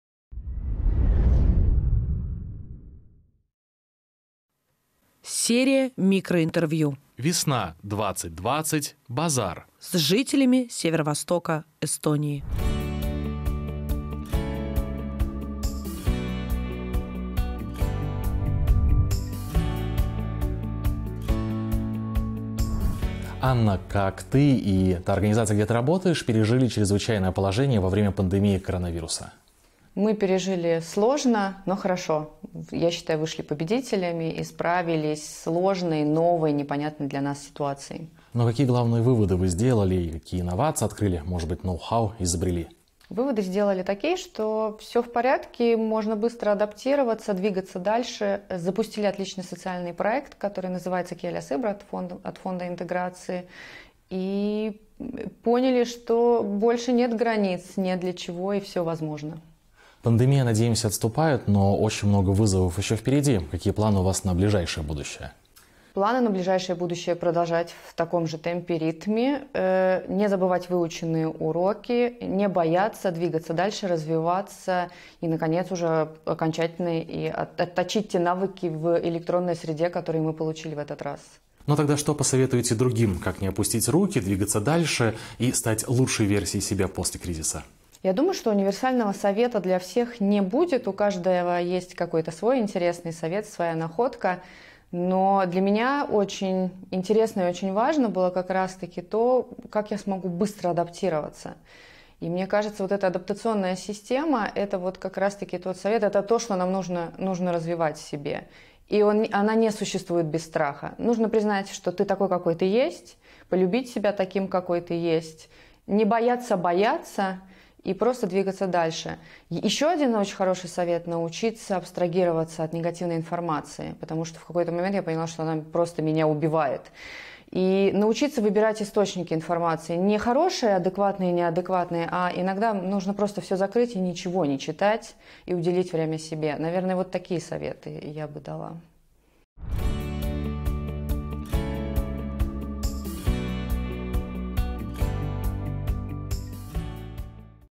VESNA2020BAZAR #4 Анна Фарафонова: «Не бояться бояться и просто двигаться дальше»
Новый проект Дня обсуждений BAZAR — серия микроинтервью, в которых жители Северо-Востока Эстонии рассказывают, как они пережили Чрезвычайное положение, и строят планы на будущее.

Герой видео: Анна Фарафонова, руководитель Нарвского дома эстонского языка.

Смотрите и комментируйте! Делитесь своими историями и этими видео.

🇪🇪 VESNA2020BAZAR #4 Anna Farafonova: “Mitte karta, vaid lihtsalt liikuda edasi”.

Uus BAZARi aruteludepäeva projekt on mikrointervjuude sari, kus Ida-Viru elanikud räägivad, kuidas nad eriolukorra üle elasid ja teevad tulevikuplaane.

Video peategelane: Anna Farafonova, Narva eesti keele maja juhataja.

Vaata ja kommenteeri! Jagage oma lugusid ja neid videoid.